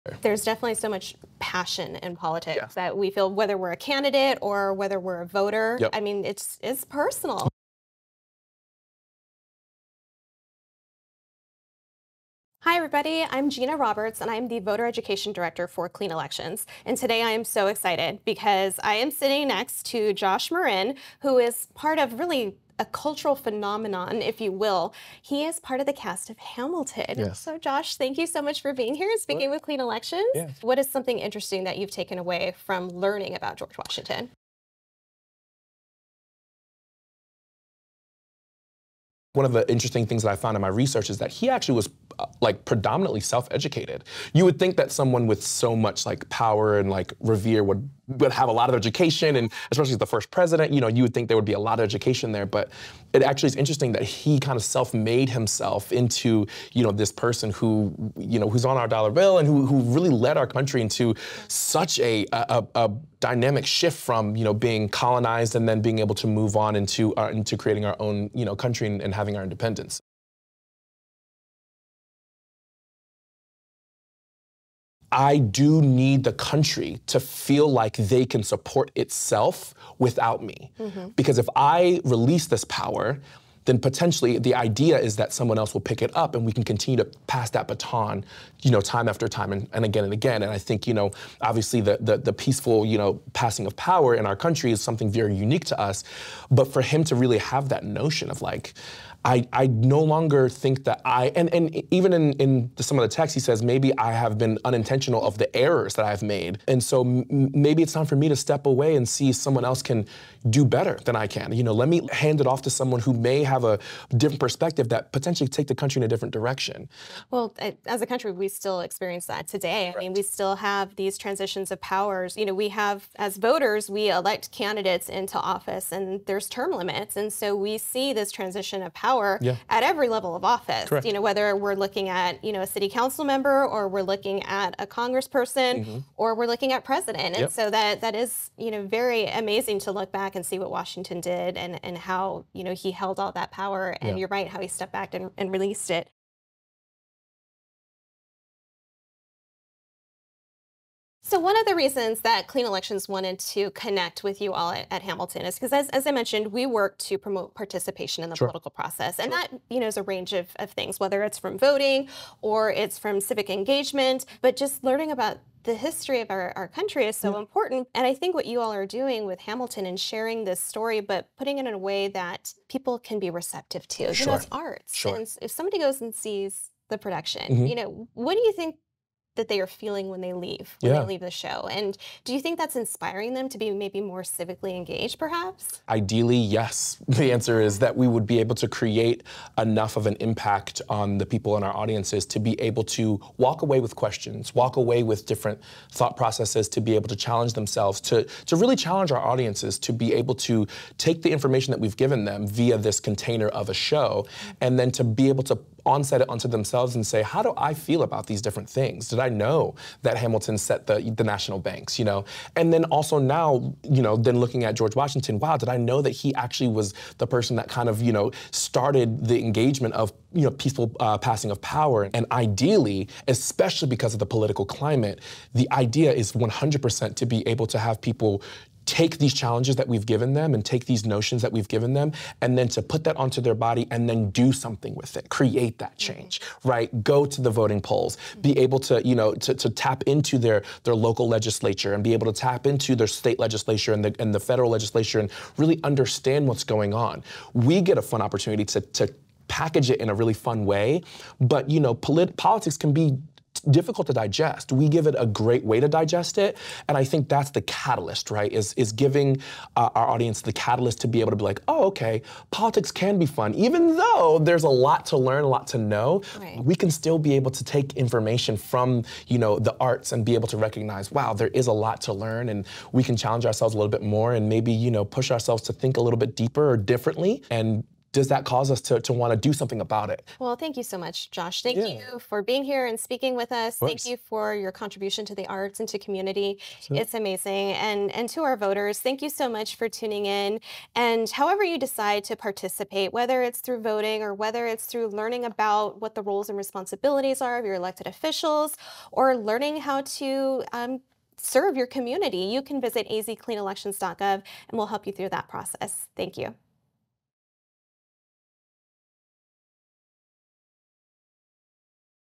Talking Democracy with the Cast of Hamilton | George Washington’s legacy & Civic Engagement
George Washington has long captivated historians as a dynamic, strong and stoic figure and founding father of the American Revolution.  Meet Josh Marin, member of the musical Hamilton cast who understudies George Washington…the hip-hop version.

Hamilton reimagines the story of the founding fathers through hip hop music and modern dancing, giving audiences a cool and unique way to learn about the history of our country.  Clean Elections sat down with the cast of the musical Hamilton to talk about how learning American history is an important part of being an informed voter, why the arts are an important tool to learn and the importance of being civically engaged.

After all, singing and dancing can be complicated, but voting doesn’t have to be. Arizona voters, visit azcleanelections.gov for information on upcoming elections, candidates and more.